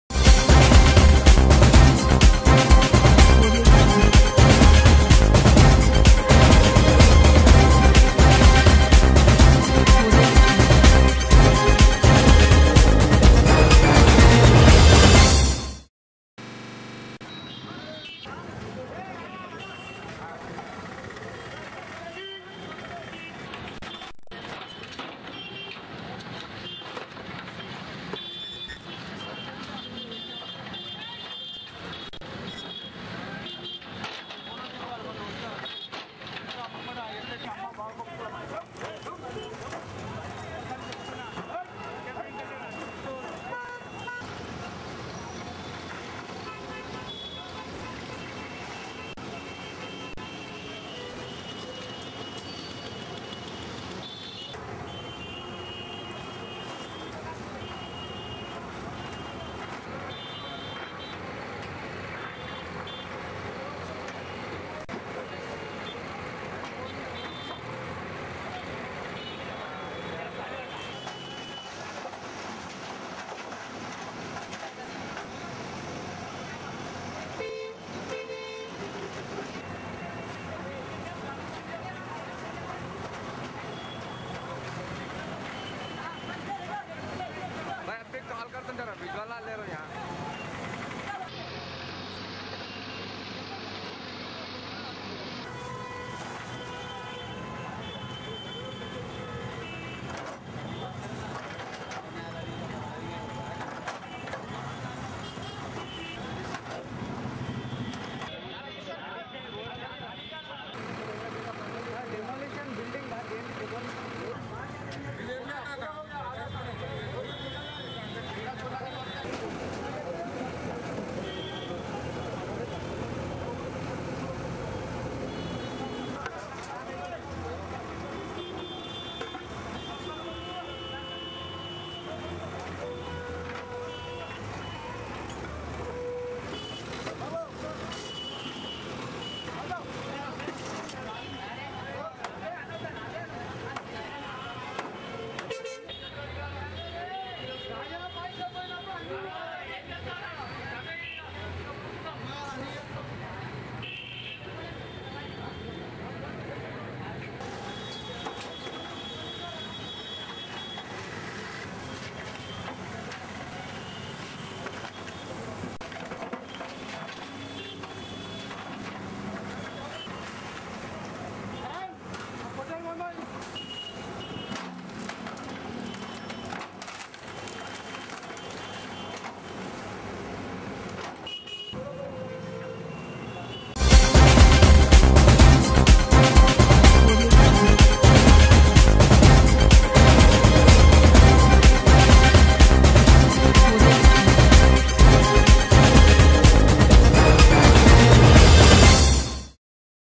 Road widening process in full swing at Sayeedabad
Road widening process in full swing at Sayeedabad. Author: Rayees Azam